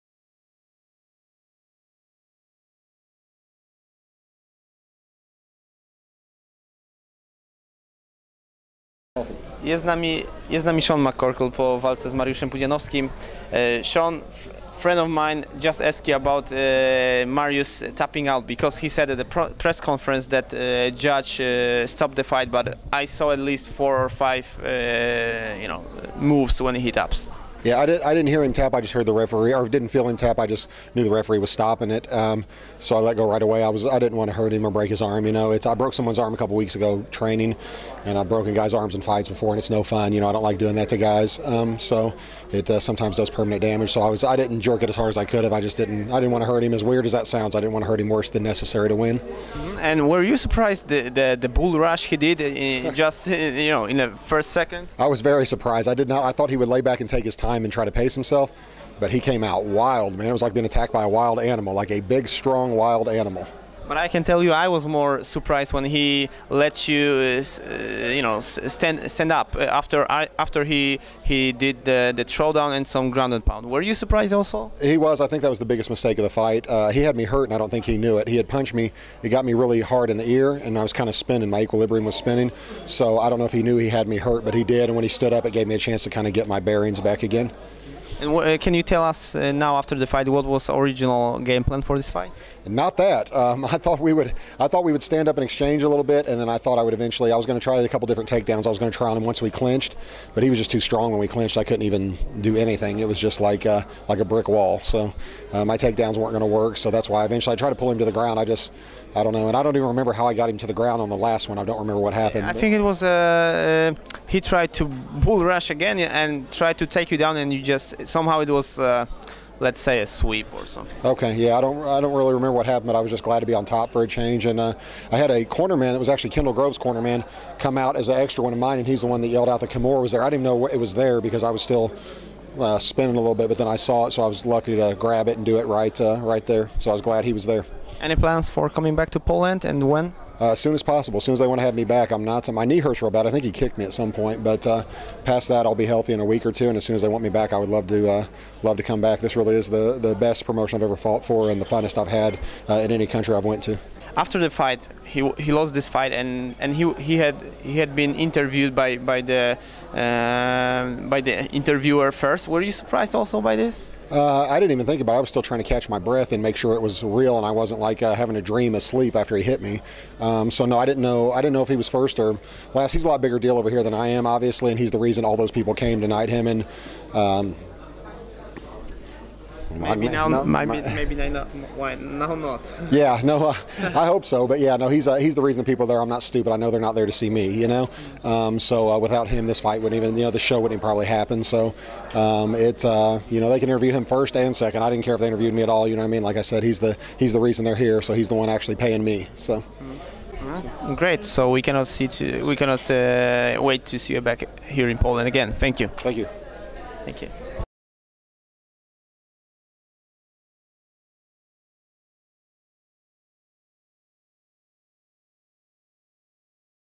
Sean McCorkle po KSW 23 - wywiad
Z przeciwnikiem Mariusza Pudzianowskiego porozmawialiśmy krótko na temat przebiegu pojedynku z Mariuszem Pudzianowskim, pytając o oczywisty game plan i błędną decyzję Pudziana o podniesieniu walki do stójki.